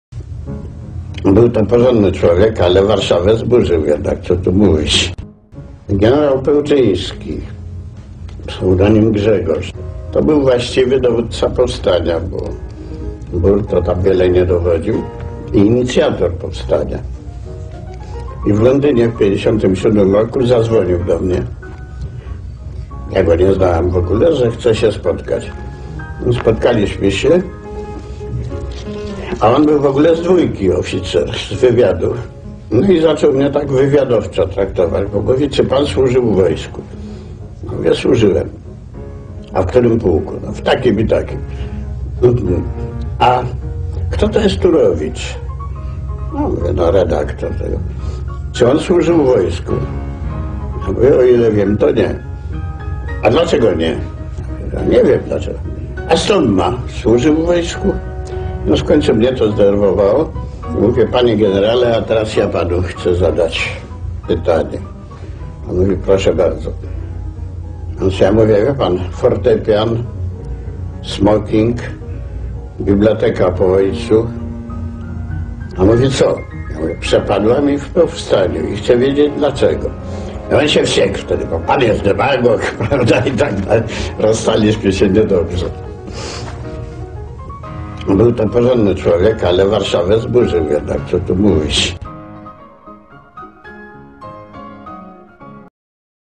Stefan Kisielewski o powstaniu warszawskim
Sylwetka generała Tadeusza "Grzegorza" Pełczyńskiego, faktycznego dowódcy powstania warszawskiego 1944 roku. Stefan "Kisiel" Kisielewski.